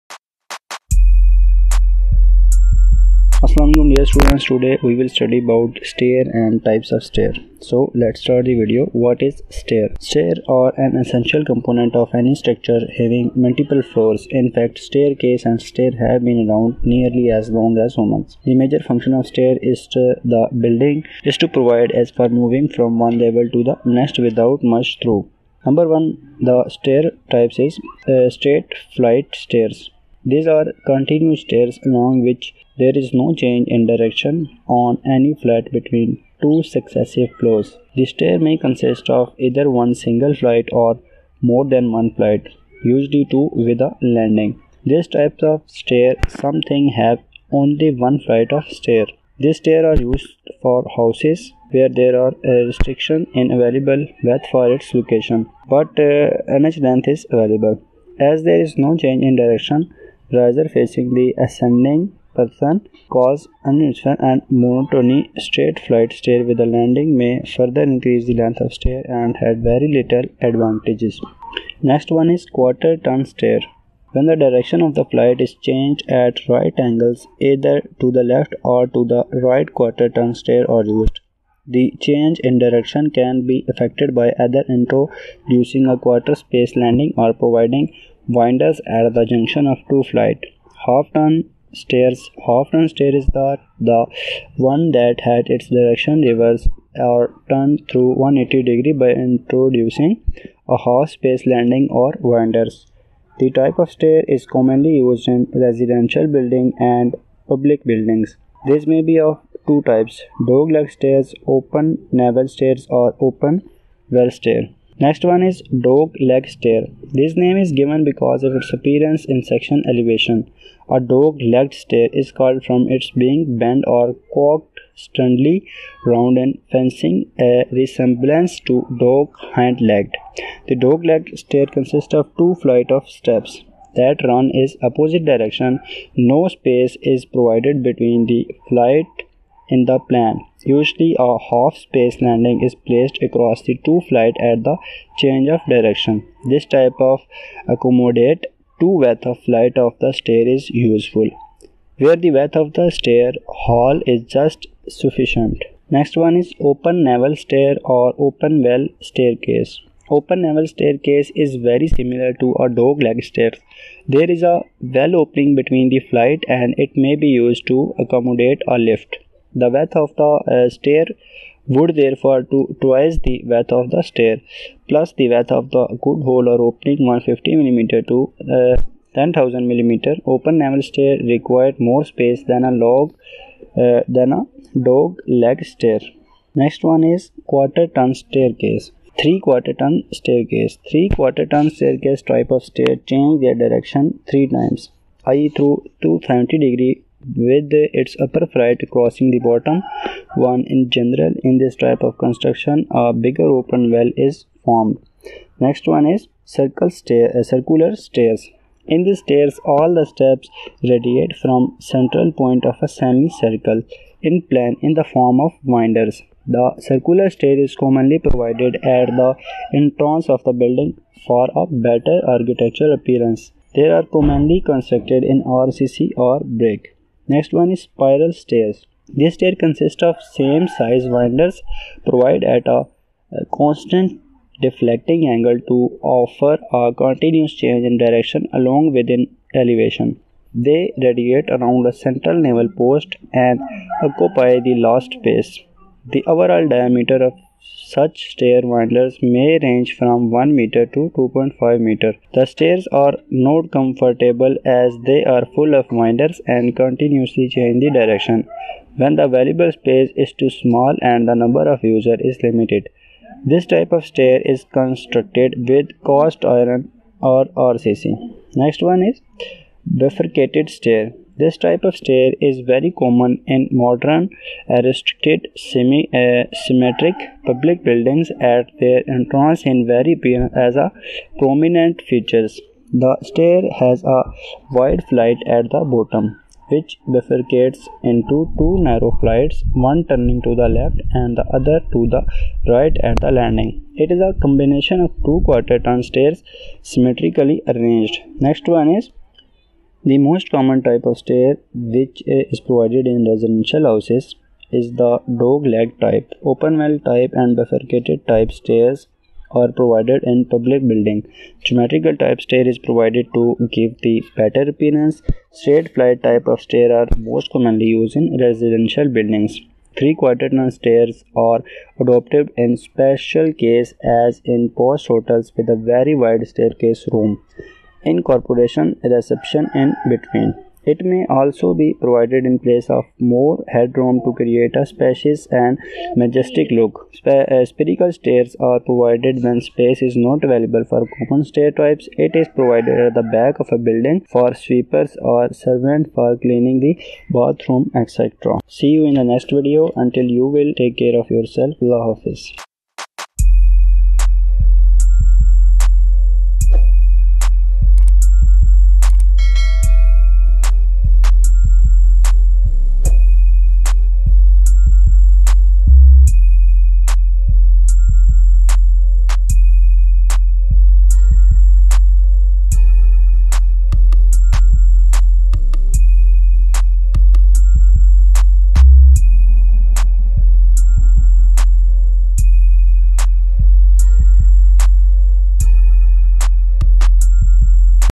Stair lecture | Types of stair | Dog leg staircase | Spiral Stairs | Straight Flight Stairs
Straight flight stairs
These are continuous stairs along which there is no change in direction on any flight between two successive floors.
The stair may consist of either one single flight or more than one flight (usually two) with a landing. These types of stairs sometimes have only 1 flight of stairs.
These stairs are used for houses where there are restrictions in available width for its location, but enough length is available. As there is no change in direction, risers facing the ascending person cause uneasiness and monotony.
Straight flight stairs with a landing may further increase the length of stairs and had very little advantage.
Quarter Turn Stairs
When the direction of the flight is changed at right angles either to the left or to the right, quarter-turn stairs are used. The change in direction can be affected by either introducing a quarter space landing or providing winders at the junction of two flights.
Half turn stairs: Half turn stair is the one that has its direction reversed or turned through 180° by introducing a half-space landing or winders. This type of stairs is commonly used in residential buildings and public buildings. These may be of two types:
a) Dog-legged stairs b) Open newel stairs or Open well stairs.
3. Dog-Legged Stairs
This name is given because of its appearance in sectional elevation. A dog-legged stair is called from its being bent or crooked suddenly round in fancied resemblance to dog’s hind leg.
The dog-legged stairs consist of two flights of steps that run in opposite directions. No space is provided between the flights in the plan.
Usually, a half-space landing is placed across the two flights at the change of direction. This type to accommodate two widths of flights of the stair is useful where the width of the stair hall is just sufficient